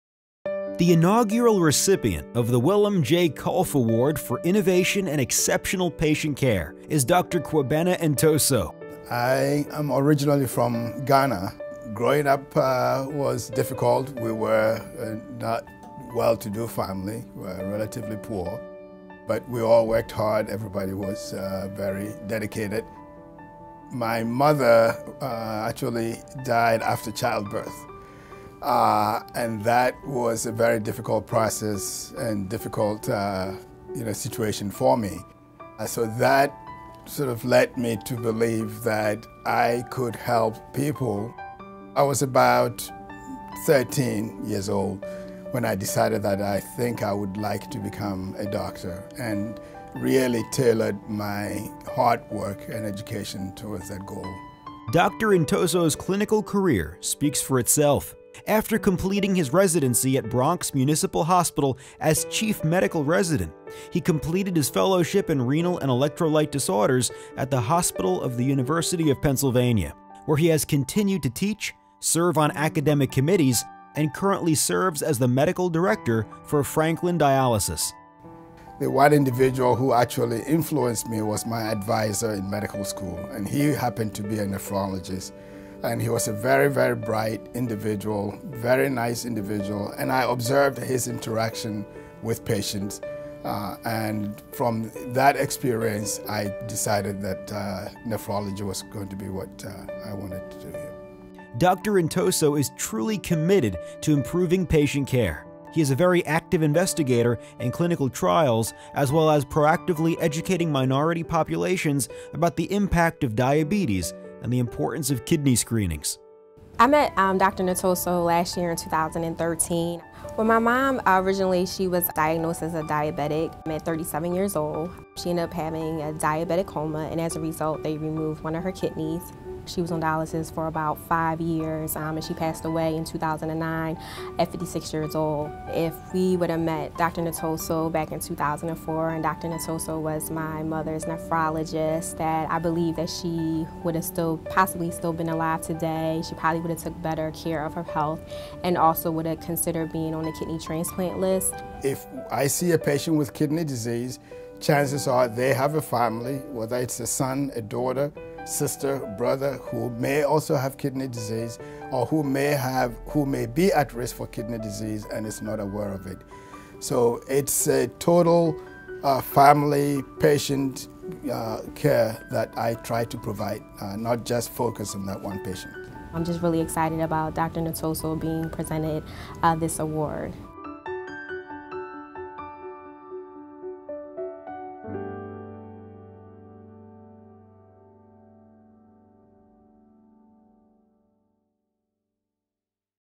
Dr. Willem J. Kolff Award Winner: Dr. K. Adu Ntoso
The DPC Education Center bestowed the first "Willem J. Kolff Award for Innovation and Exceptional Patient Care" to Dr. K. Adu Ntoso of Philadelphia. Surrounded by his family, he was in Washington DC to accept the award from Dr. Jack Kolff, the eldest son of the late Dr. Willem Kolff.